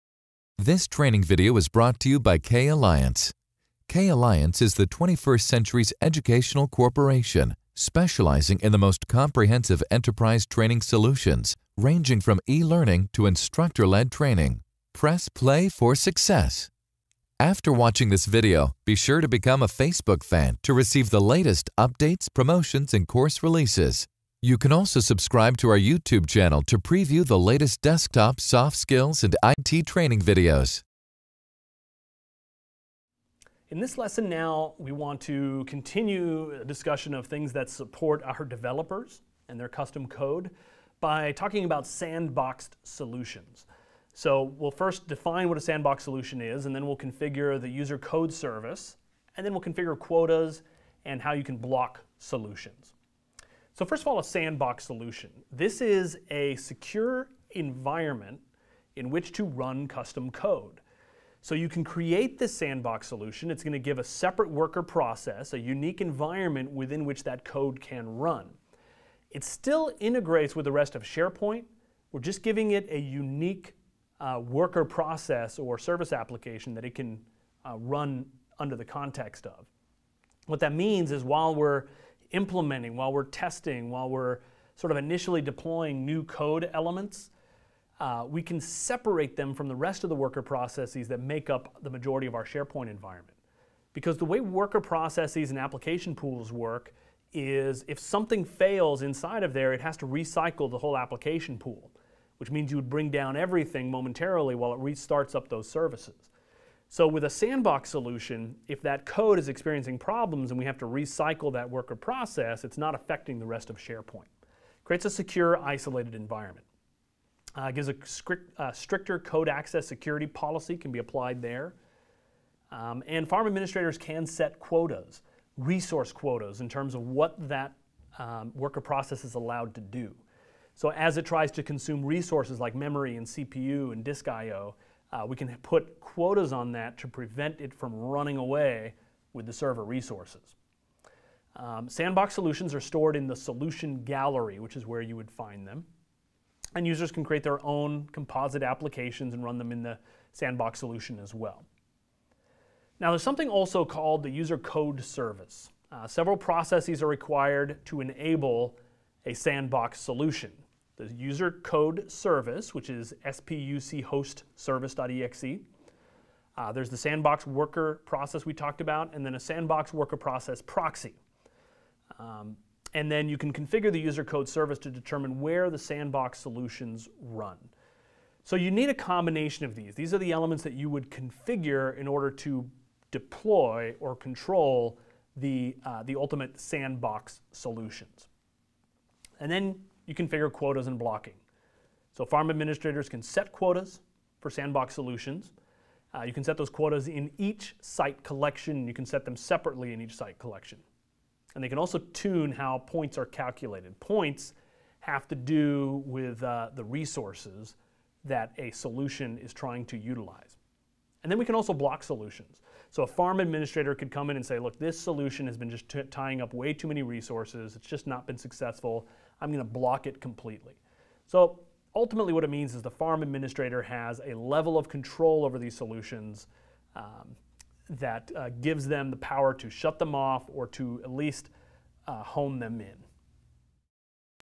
Microsoft SharePoint Server 2010 Tutorial: Configuring Sandboxed and Solutions | K Alliance
Learn how to configure sandboxed solutions with this Microsoft SharePoint Server 2010 training video. Contact K Alliance for more info. - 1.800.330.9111

Featured Instructor - Doug Pyle
Course Duration: 11 Hours 56 Minutes